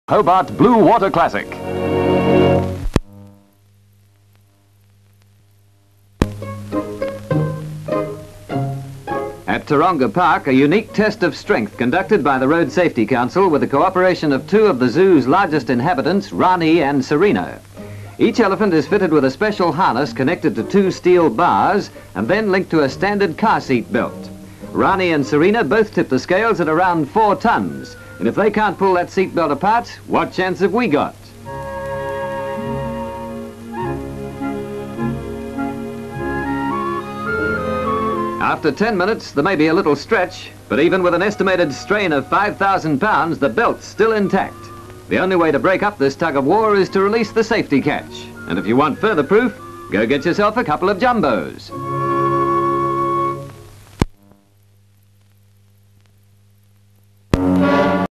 ELEPHANTS TEST SEAT BELTS - SOUND
Various shots at Taronga Park, showing a unique test of strength conducted by the Road Safety Council with the co-operation of two of the zoo's largest inhabitants. Each elephant was fitted with a special harness, connected to two steel bars and then linked to a standard car seat belt. Both elephants weigh about four tons, and if they can't break the belt, it is naturally considered safe in a car.